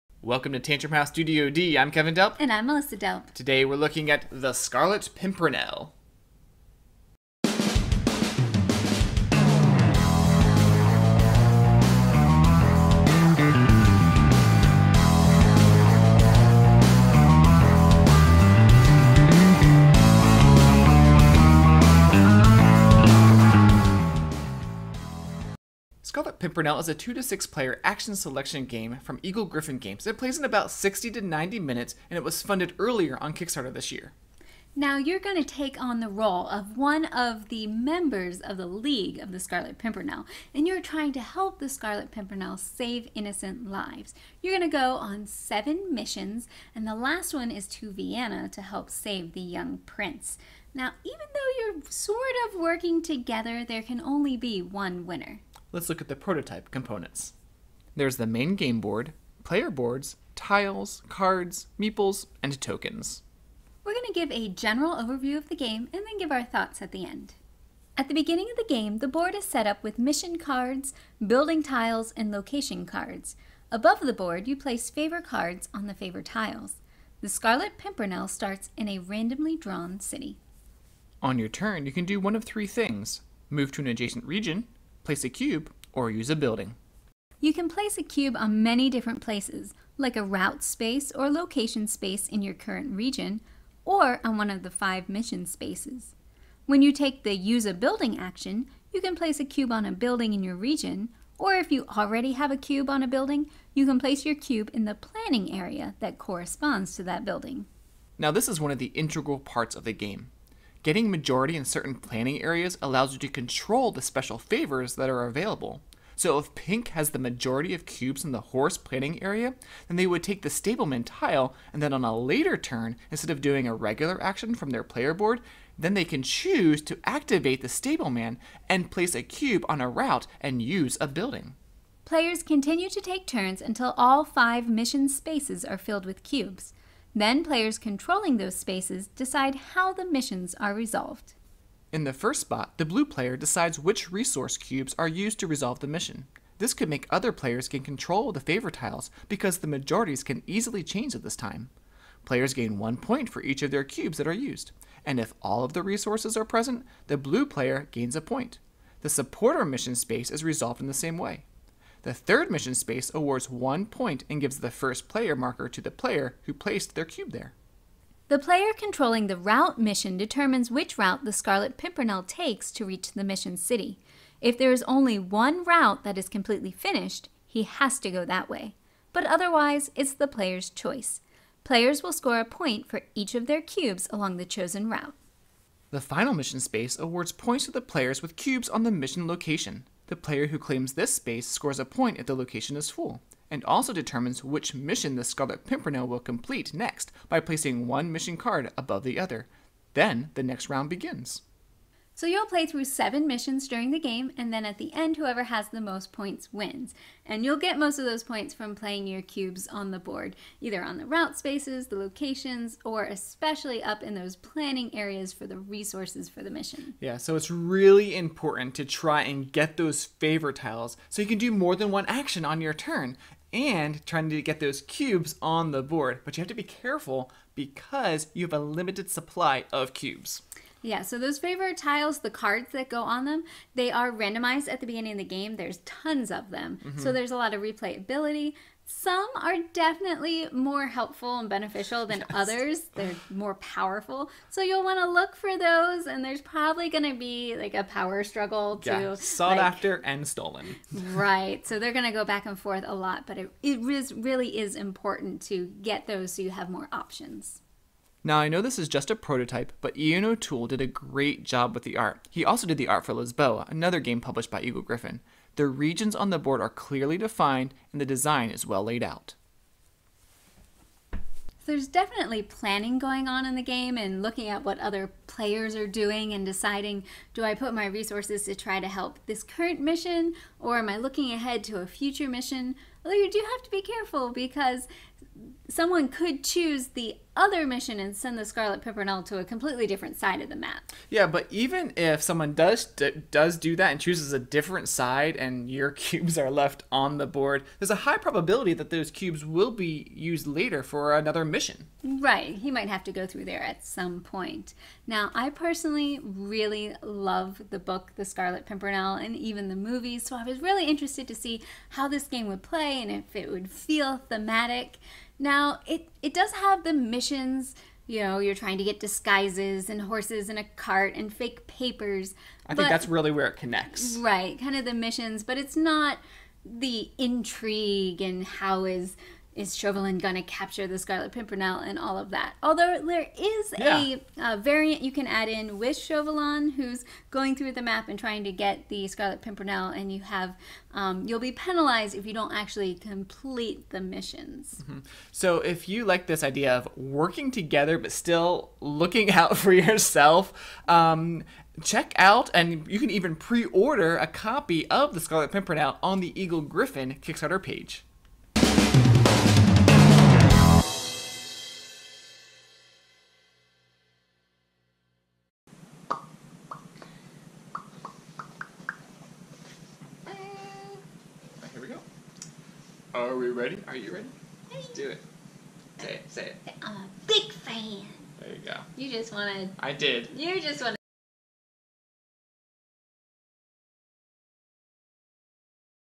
Scarlet Pimpernel Board Game Preview
Kevin and Melissa look at a prototype of The Scarlet Pimpernel being published by Eagle Gryphon Games.